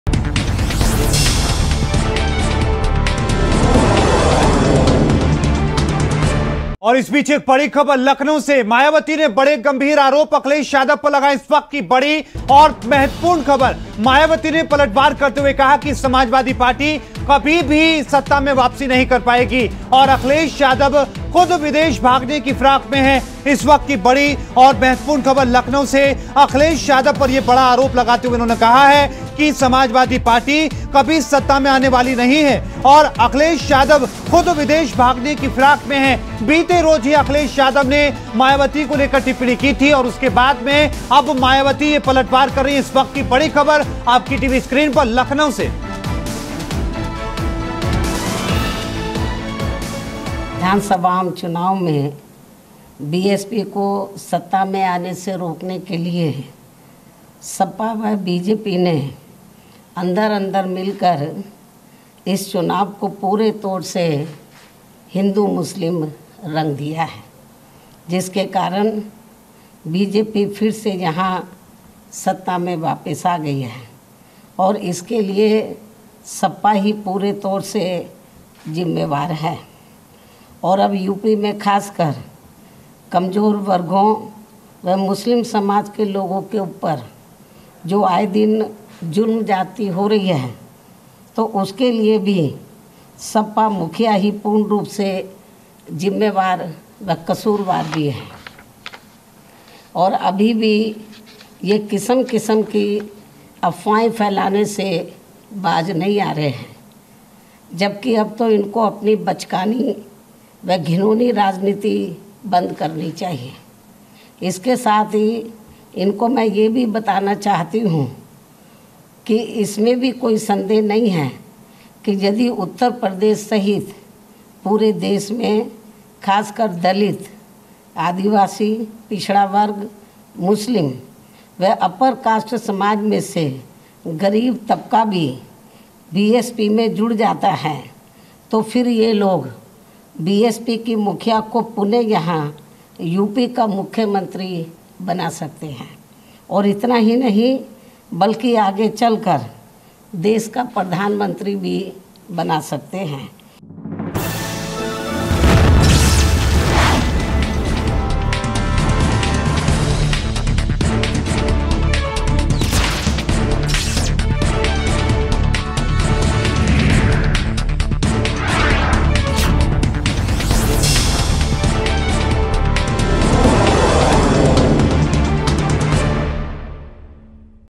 'Akhilesh Yadav विदेश भागने की फिराक में हैं’ - Mayawati ने Akhilesh Yadav पर लगाए आरोप | R9 News
अखिलेश यादव पर मायावती का पलटवार
सपा कभी सत्ता में नहीं आने वाली- मायावती
‘अखिलेश यादव विदेश भागने की फिराक में हैं’
मायावती ने अखिलेश यादव पर लगाए आरोप
बीते रोज ही अखिलेश यादव ने किया था हमला